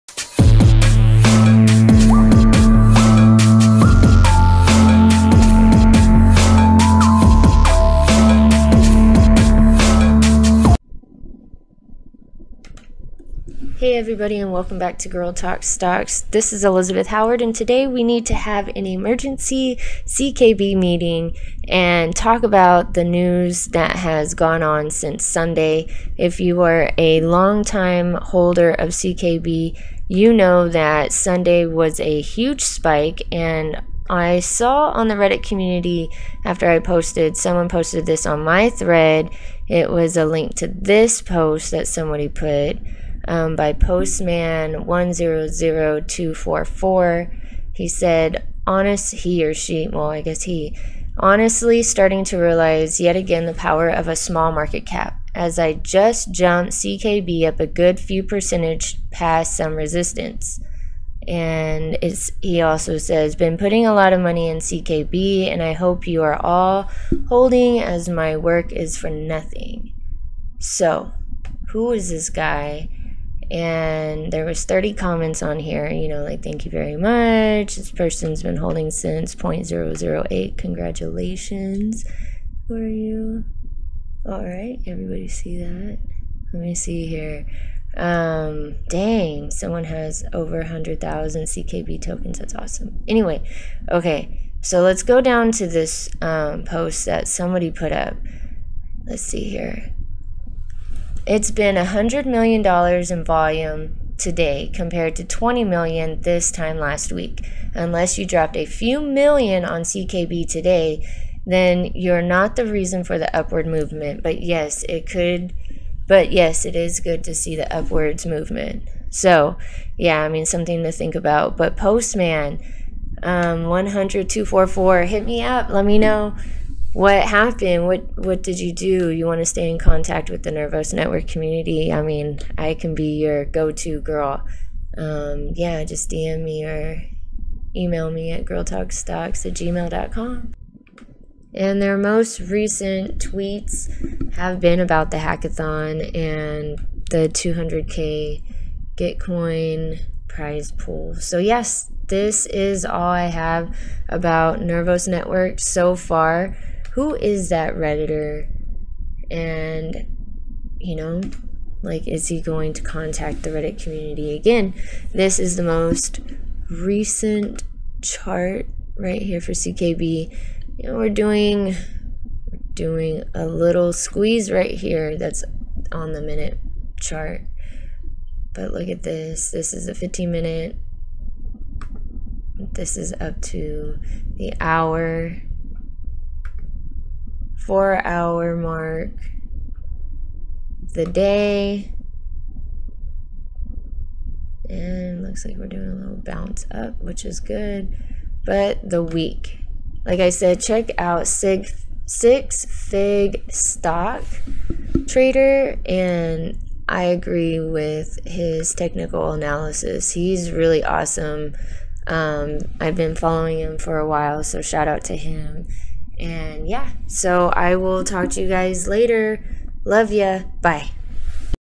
Nervos Network News Update 08/25/21 Mysterious Redditor
What was the reason for last Sunday's pump? Is it the hackathon? Is it this mysterious redditor?   Shout out to the Nervos Network Reddit community and don't forget to check out 6figstocktrader here on Youtube for his technical analysis that I 100% agree with!  Nervos Network is the Future and now is the time to strike!